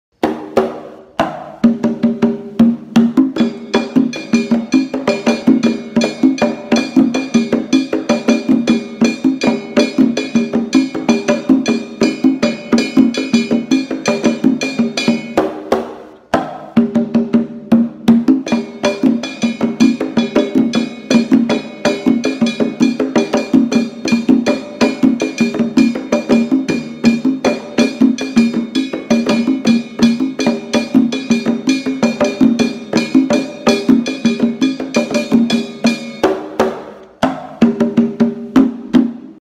Cabila kabula
O Workshop com o músico  @davidmarianomariano ofereceu ao público uma visão geral sobre os principais instrumentos e formas da linguagem Latina, africana e brasileira, terá como finalidade ensinar as técnicas, ritmos, independência, leituras, claves, ostinatos,  entre outros,  desenvolvendo  um trabalho de percepção rítmica, prática de conjunto e  técnicas modernas.
Aconteceu nos dias 28,29 e 30 de Dezembro das 18h às 21h